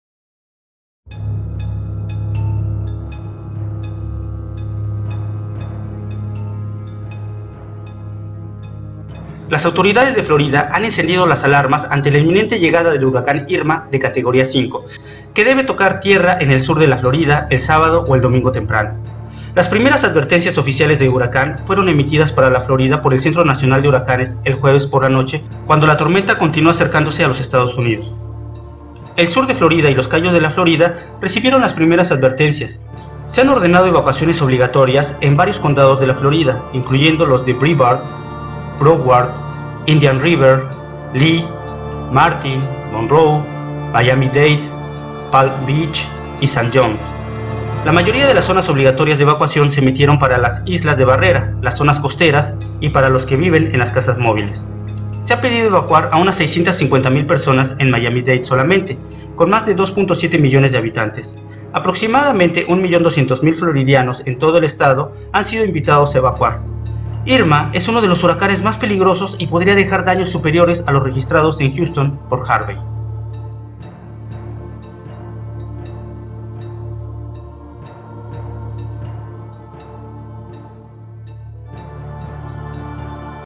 Huracán Irma impactará Florida el fin de semana
HuracánIrma y su poder destructivo están por llegar a Florida, ante ello, las autoridades de ese estado han activado las alarmas y han invitado a evacuar a por lo menos a 1 millón 200 mil floridanos. Huracán Irma es categoría 5.